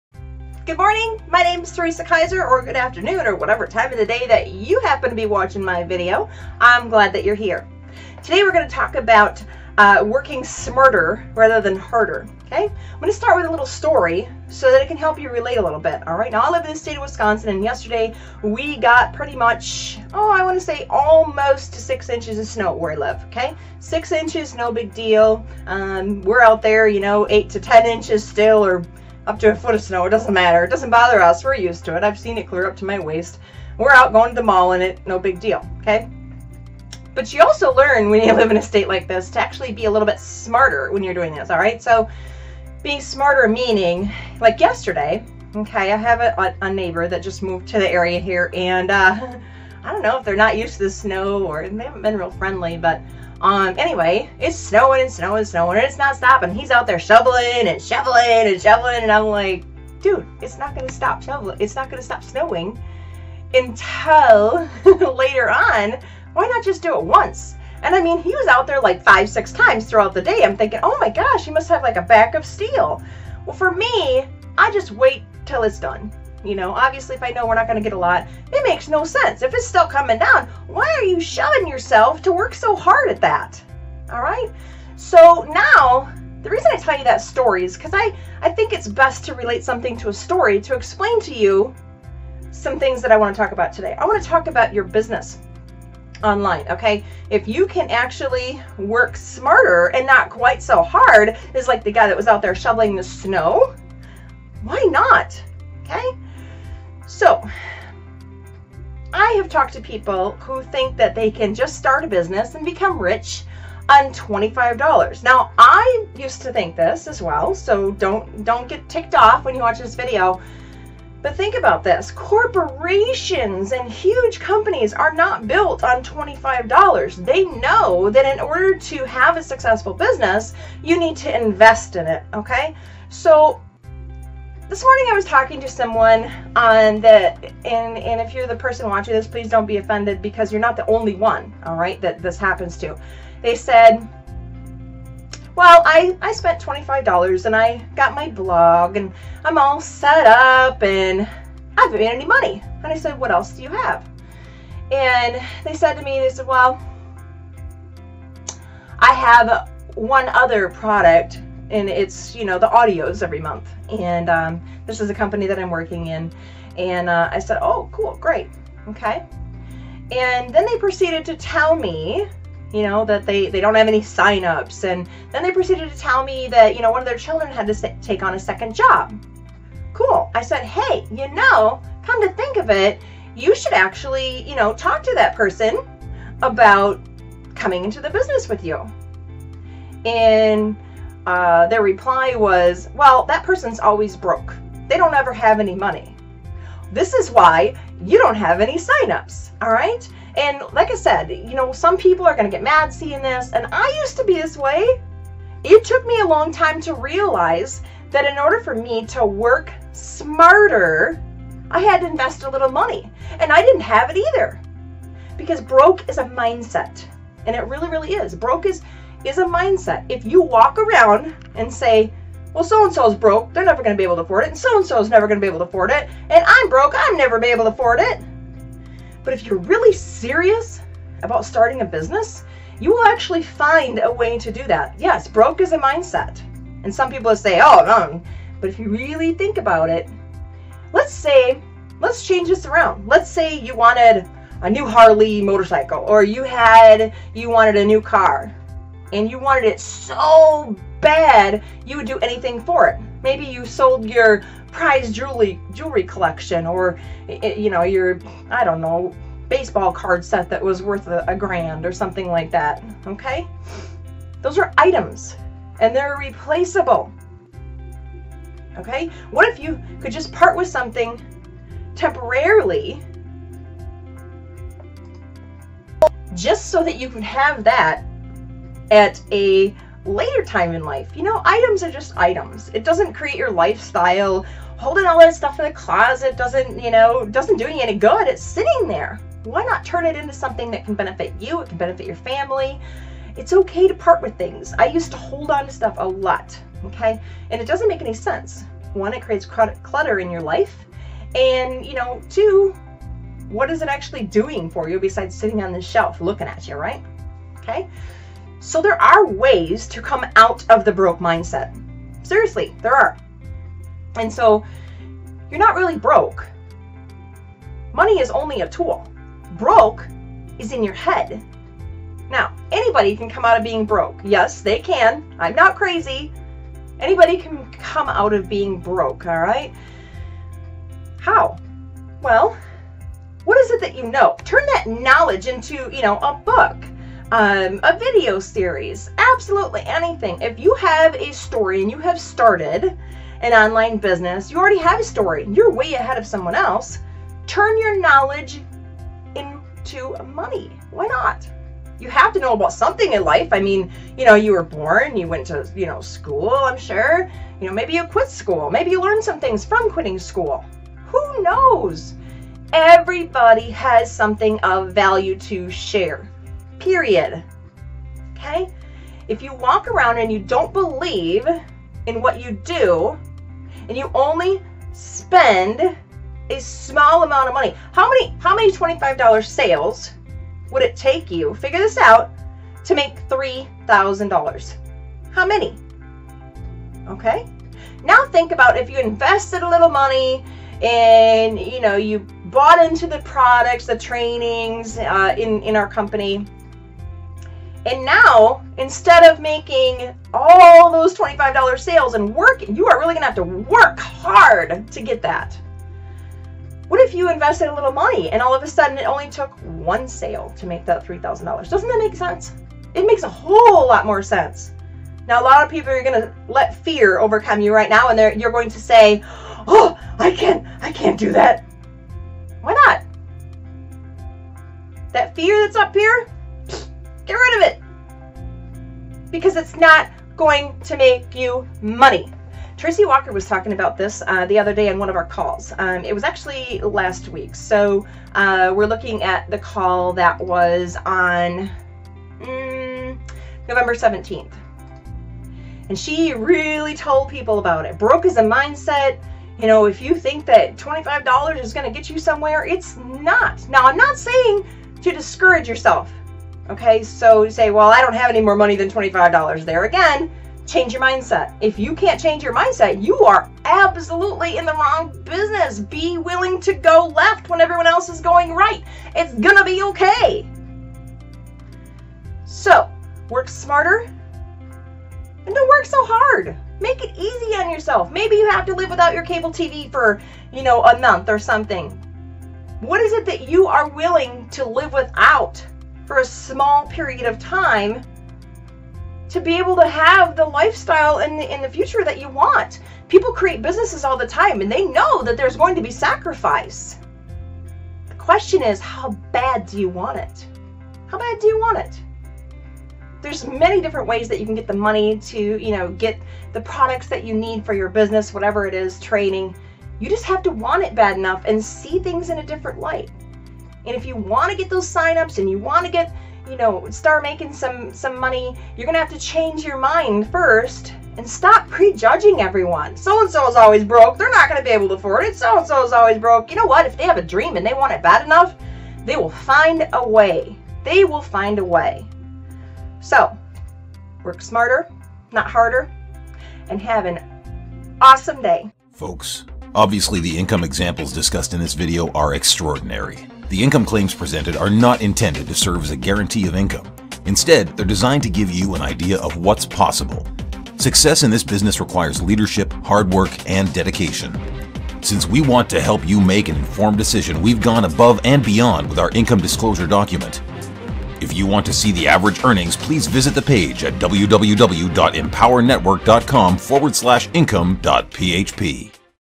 Your Way of Thinking Could Be Holding You Back!
Are you spending ALL day working on your business?

Do you keep telling yourself you can't afford things?

Are you holding onto things of value. why? You can't take them with you when you leave this world!

Life is MEANT to be lived to the fullest. why aren't you doing it, and enjoying your time in this life?

As I always say...tomorrow is never promised to anyone.